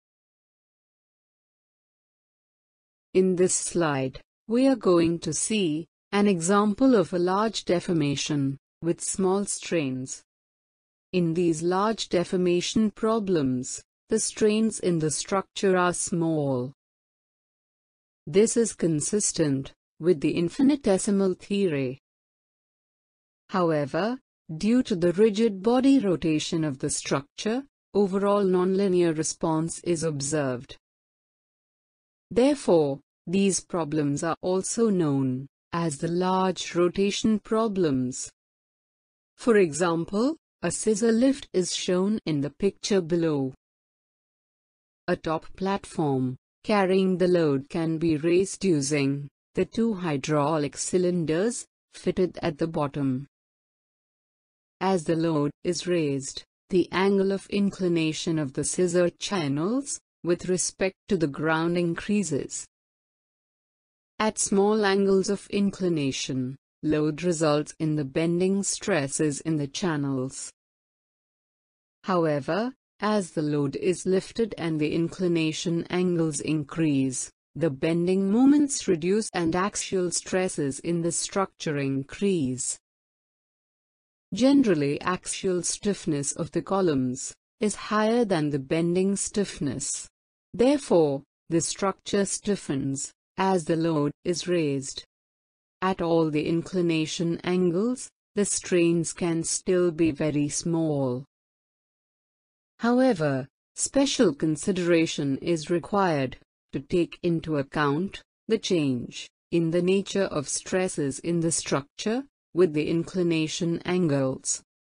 Large Deformation
This video explains what a large deformation or rotation condition in the context of Geometric Nonlinearity is.